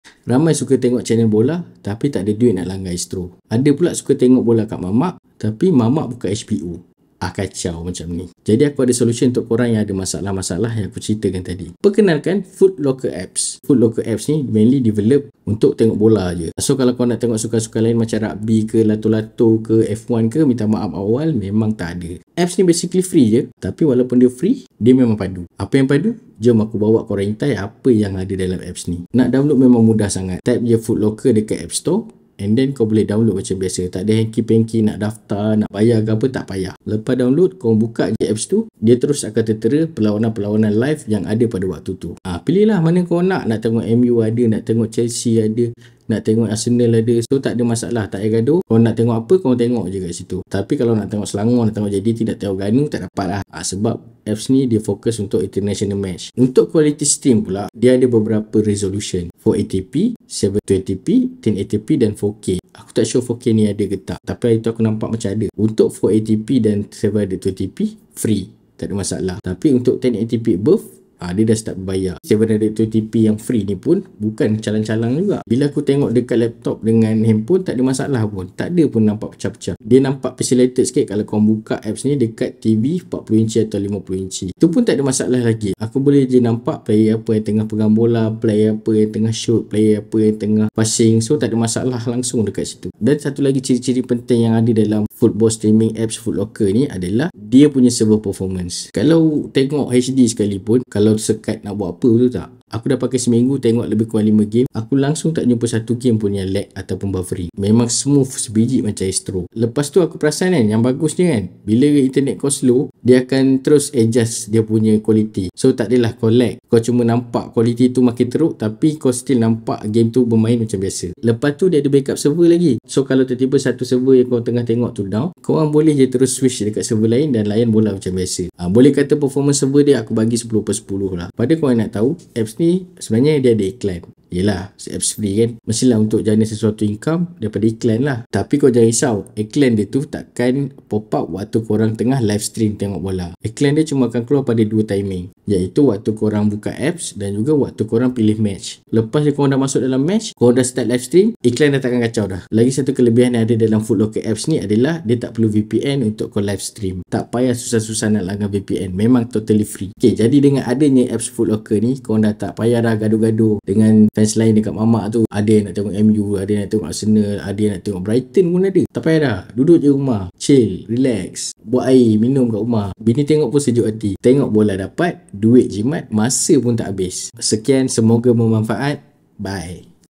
Apps Tengok Bola Free| EPL, La Liga, UCL, Europa, Copa America & Sebagainya | Football Rocker Apps
Apps ni memang wajib korang try. Dijamin tak mengecewakan. Mudah sangat nak download dan pakai. Semoga bermanfaat 🫶🏻